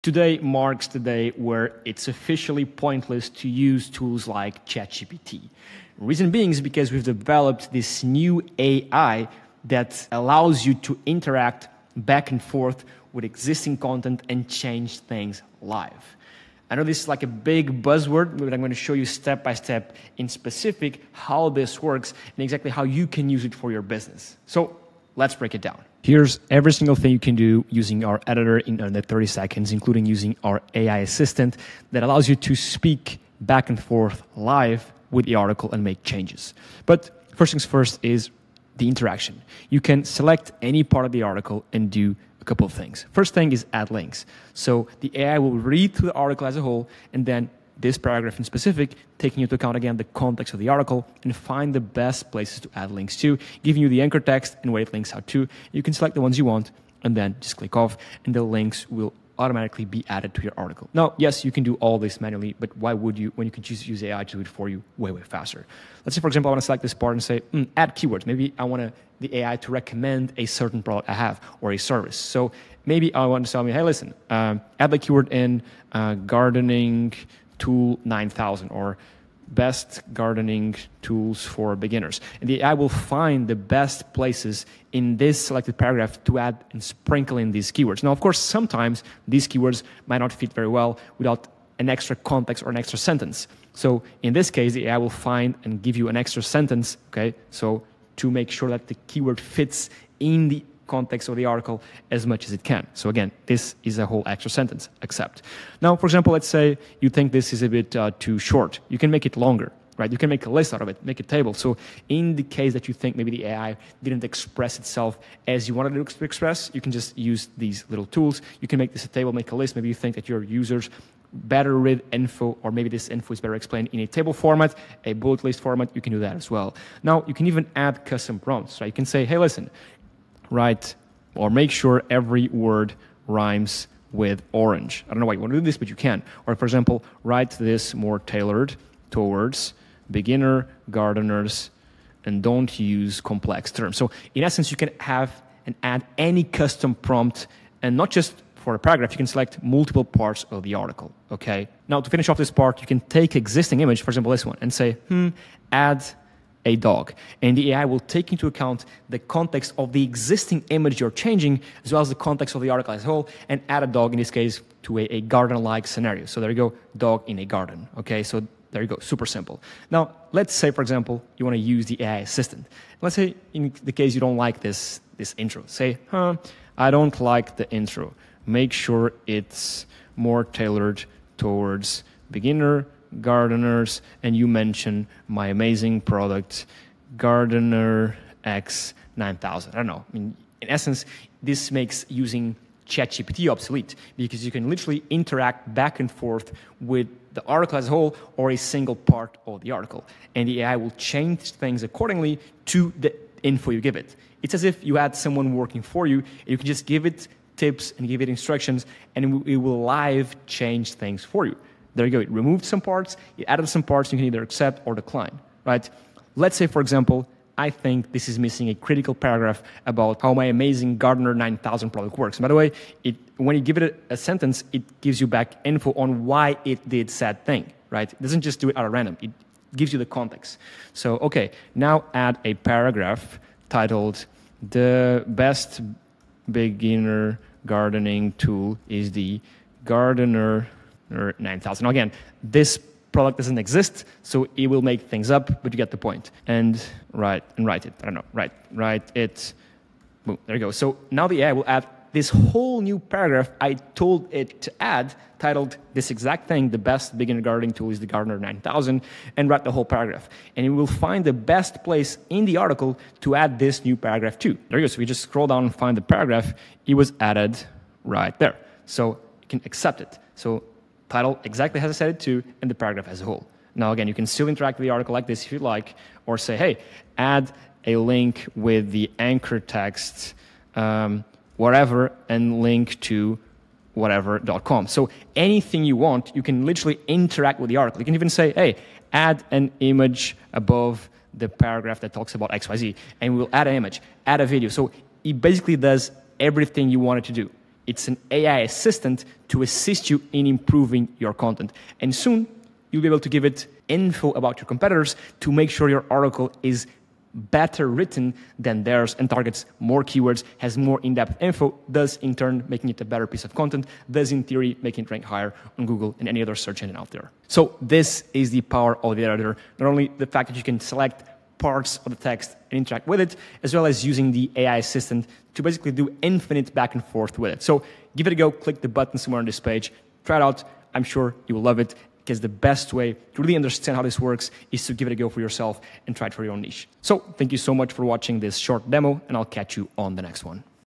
This AI SEO Content Editor makes ChatGPT Useless
Try it Stop using ChatGPT for SEO, use this AI Content Editor and 100x your speed.

In this video, I show you why traditional AI tools like ChatGPT are becoming outdated—and how our new editor completely changes the game. You’ll see a full demo of how you can interact live with your content, update articles on the fly, add keywords, links, images, and even give complex prompts like “rewrite for beginner gardeners” or “add a product paragraph.”

This tool acts like a real-time AI assistant that edits your content based on your instructions—no more switching between tabs or waiting on prompts. Watch how I use it to turn a basic article into SEO-optimized content in just minutes. Perfect for marketers, bloggers, and anyone serious about content performance.